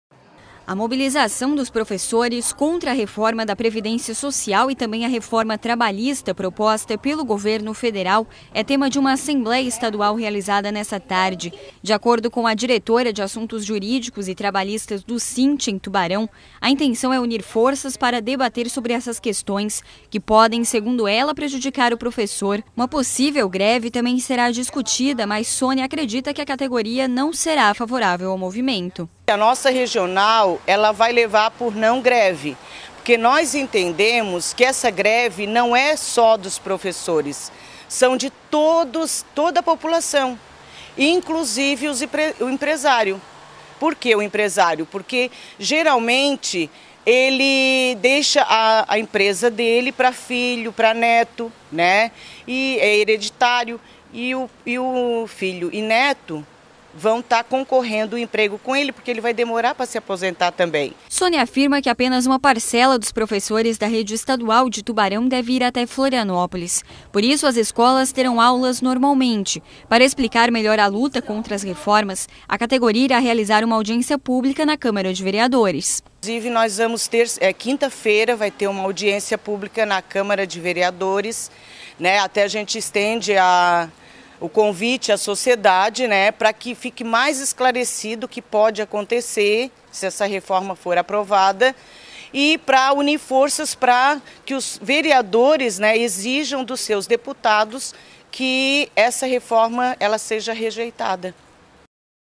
Sinte realiza Assembleia Estadual em Florianópolis
O Sindicato dos Trabalhadores em Educação da Rede Pública de Ensino do Estado de Santa Catarina, o Sinte, faz uma Assembleia Estadual nessa terça-feira, 28/3, em Florianópolis, para decidir ou não por uma possível greve da categoria. Entre os assuntos que vão ser debatidos, estão as reformas da Previdência Social e Trabalhista, proposta pelo Governo do Estado